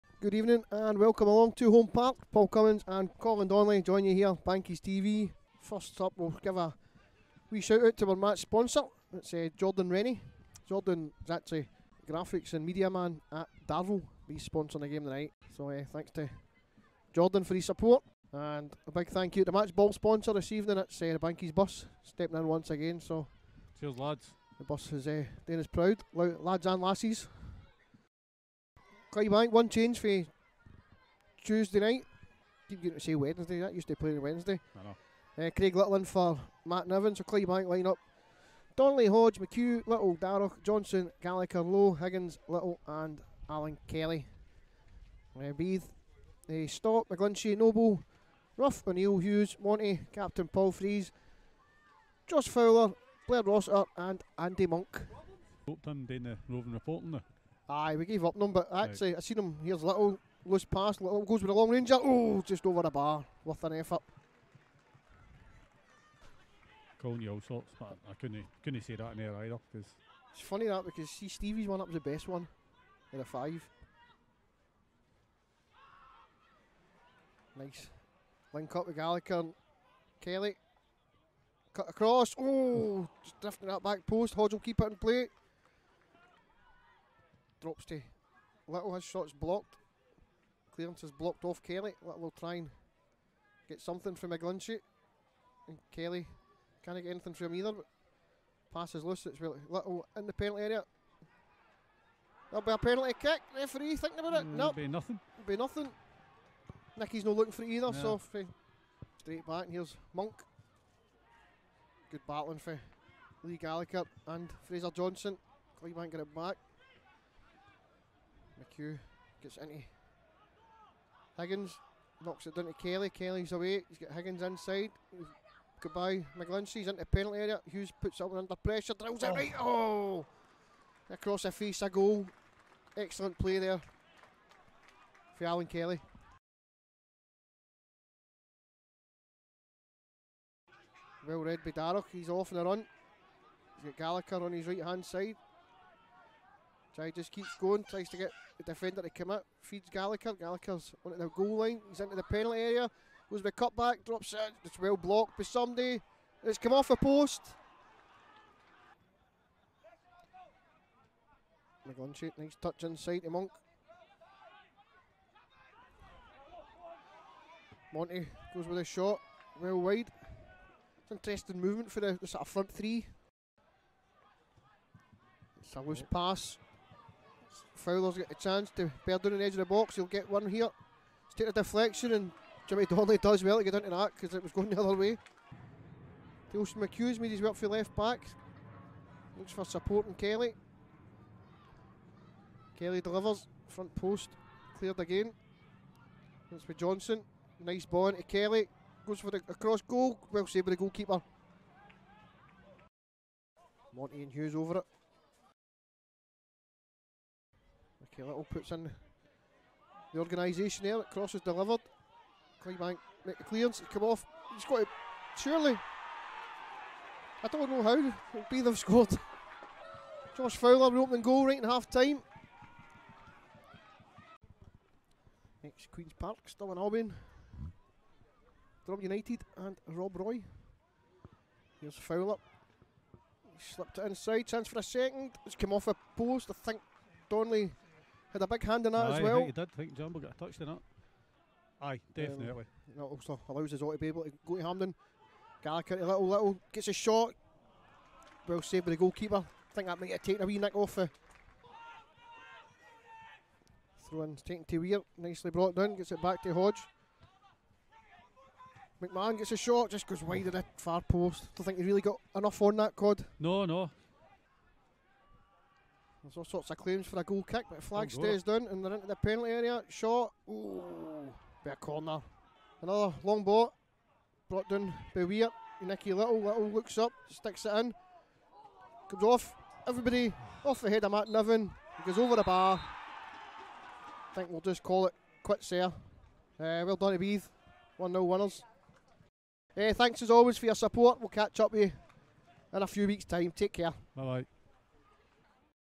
HIGHLIGHTS: Clydebank 0-1 Beith
Highlights of Clydebank's 1-0 defeat to Beith at Holm Park in the West of Scotland Premier Division,